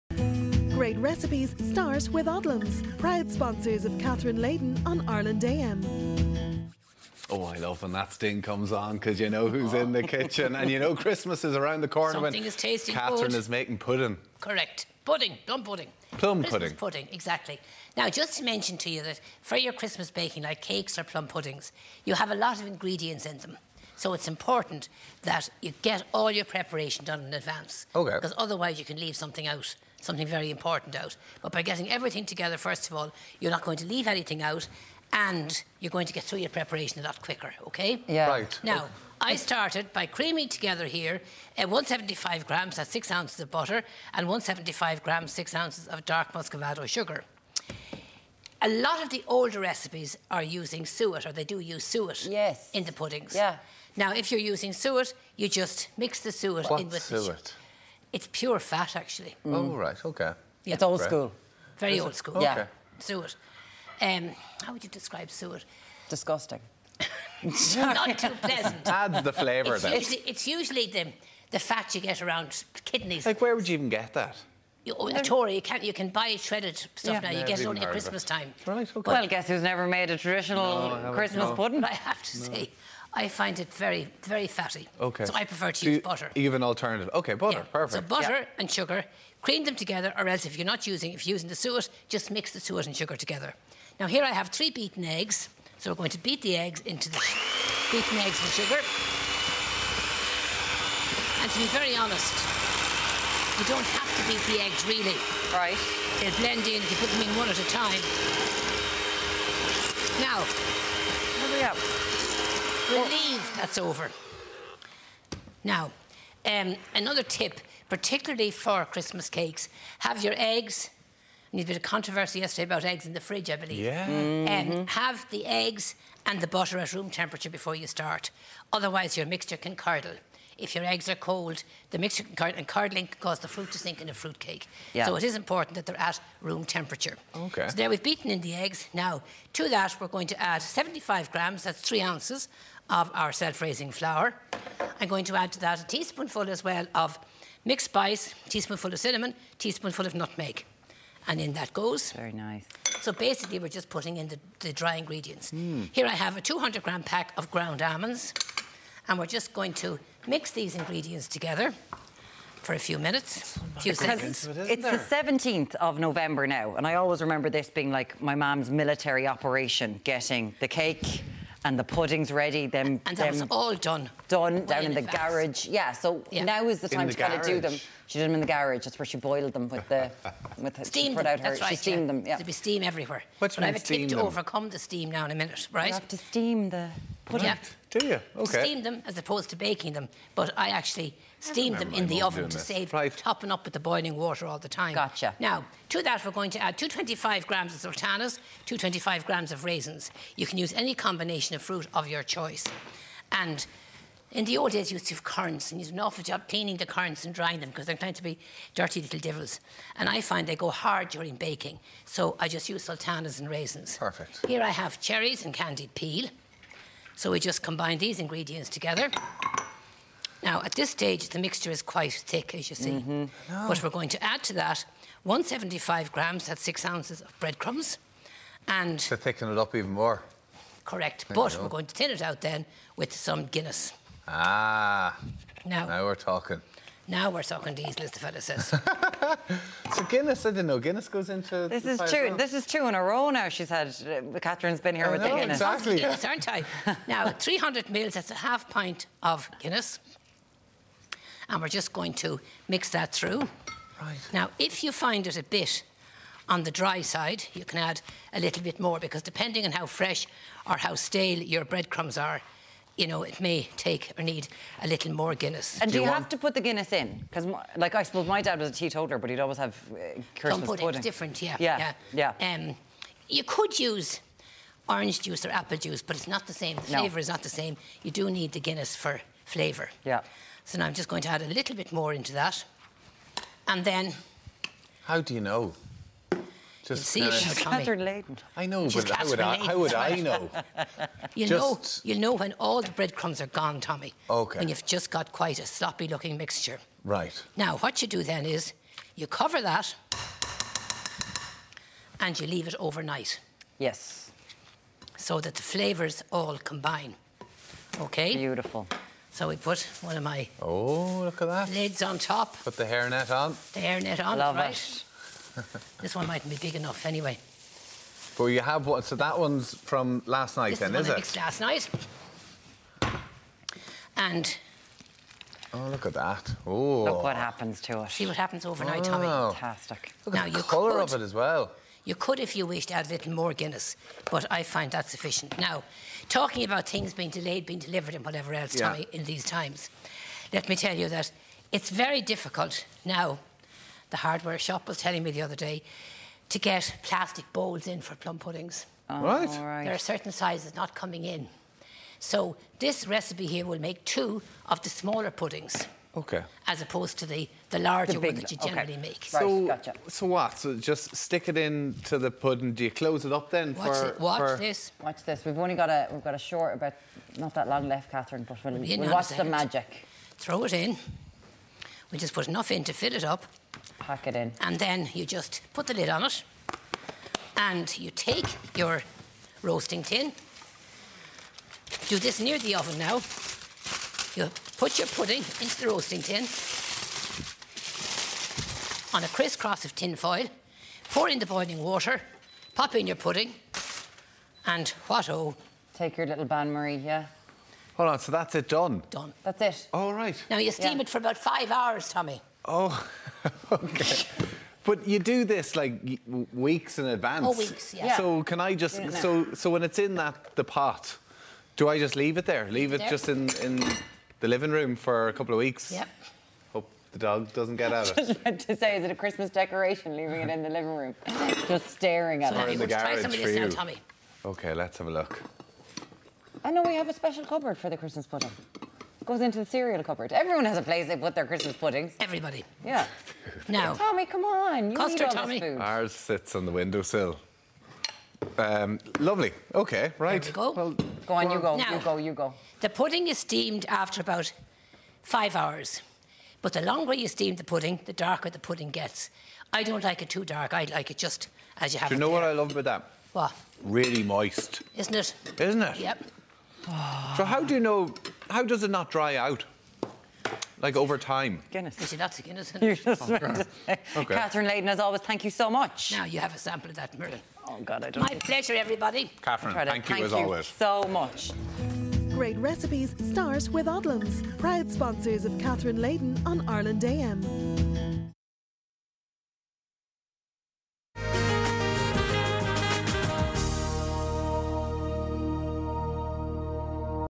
How to Make Christmas Pudding | Odlums
How to Make Christmas Pudding | Odlums. Here is how Catherine Leyden makes her delicious Christmas Pudding.

Odlums The official YouTube channel of Odlums.